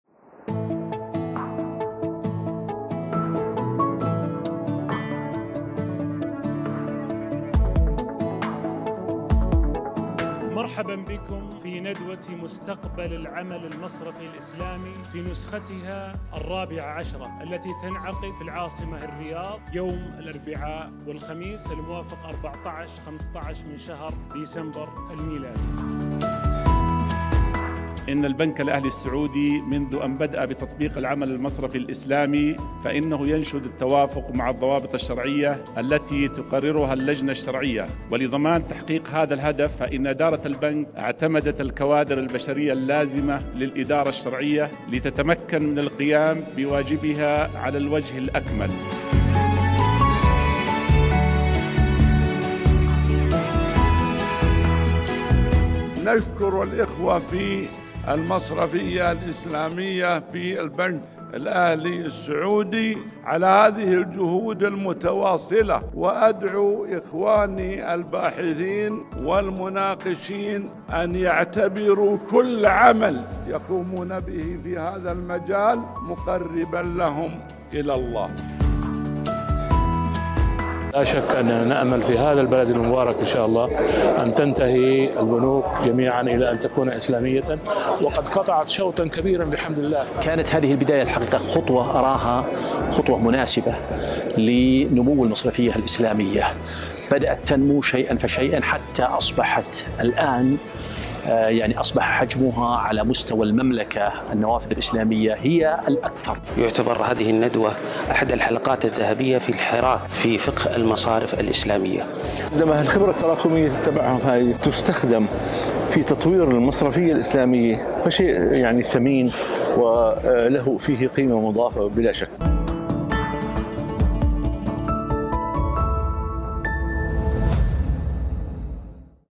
فعاليات ندوة_مستقبل_العمل_المصرفي_الإسلامي
فعاليات ندوة_مستقبل_العمل_المصرفي_الإسلامي تحت عنوان "البنوك ذات النوافذ الإسلامية" بمشاركة نخبة من كبار العلماء والمختصين بالمصرفية الإسلامية